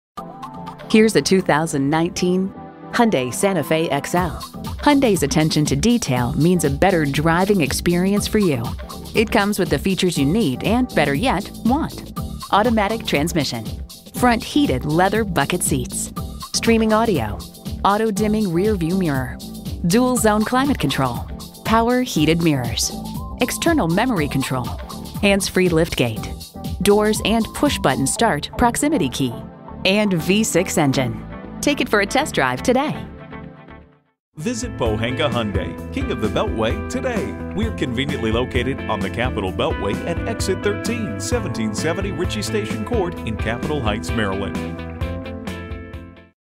Used 2019 Hyundai Santa Fe XL Capitol Heights MD Washington-DC, MD
Year: 2019
Make: Hyundai
Model: Santa Fe XL
Trim: SE
Engine: 3.3L V6 DGI DOHC 24V
Transmission: 6-Speed Automatic with Shiftronic
Color: Silver
Interior: gray
Mileage: 479
Stock #: FKU302429A
VIN: KM8SMDHF4KU305537

Address:
1770 Ritchie Station Court